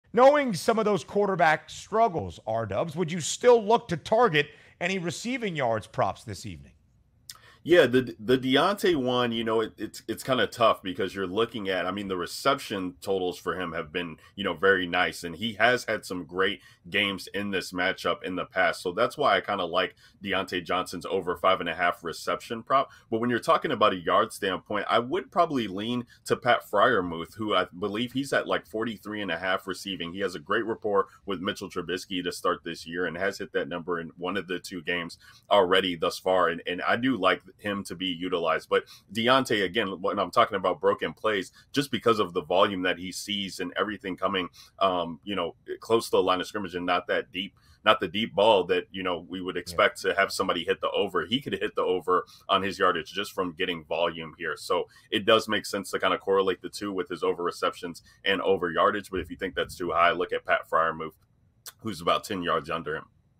NFL TNF Player Props: Wide Receiver Yard Totals
Ryan Williams joins Ben to discuss a few wide receiver yard totals for Thursday night's matchup between the Pittsburgh Steelers and Cleveland Browns.